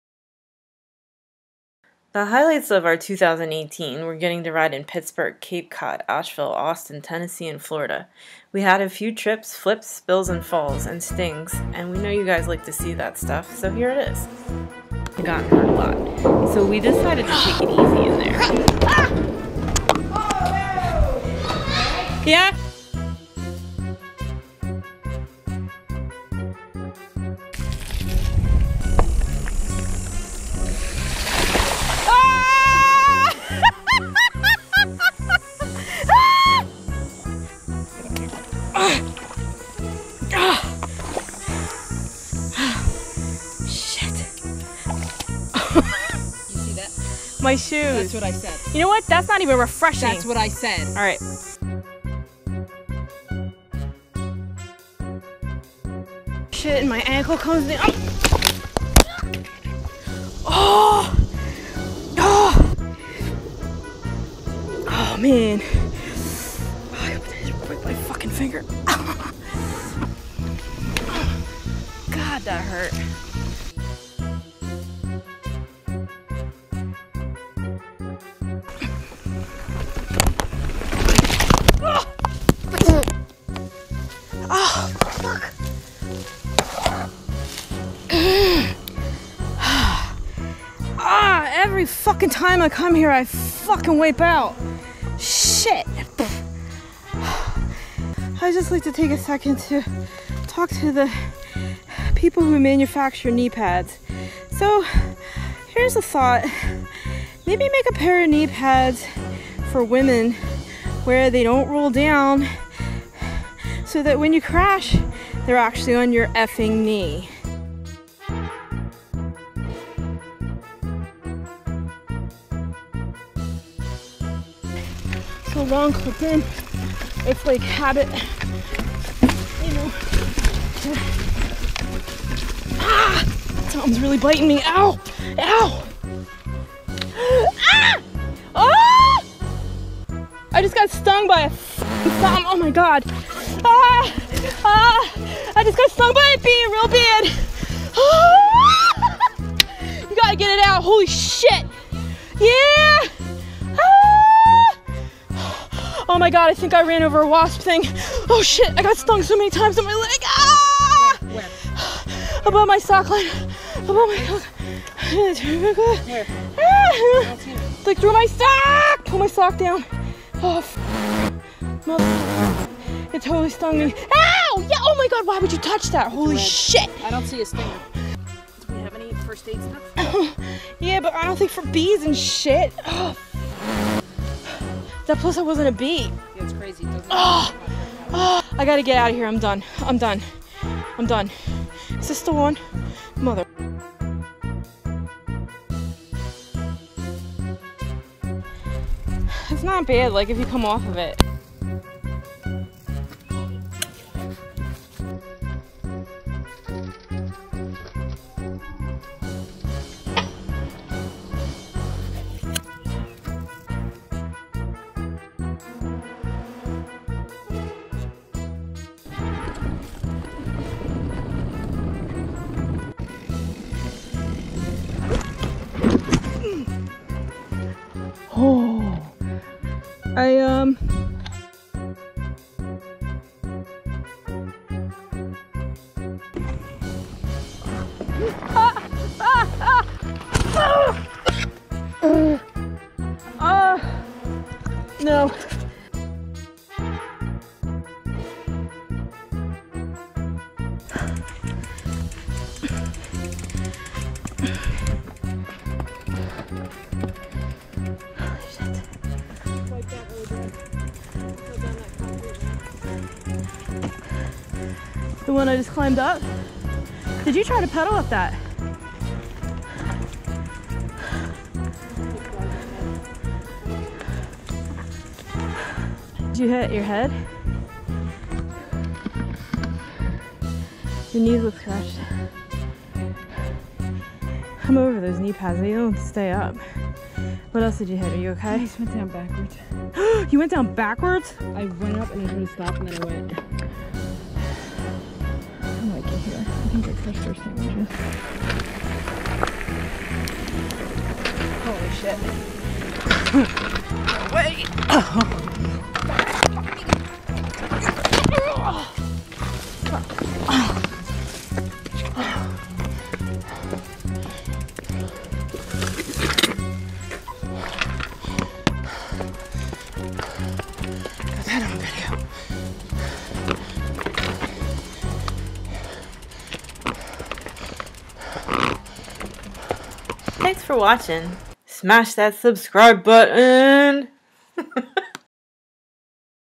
2018 MTB Crash Reel
The highlights of our 2018 were getting to ride in Pittsburgh, Cape Cod, Asheville, Austin, Tennessee, and Florida. We had a few trips, flips, spill falls and stings and we know you guys like to see that stuff, so here it is!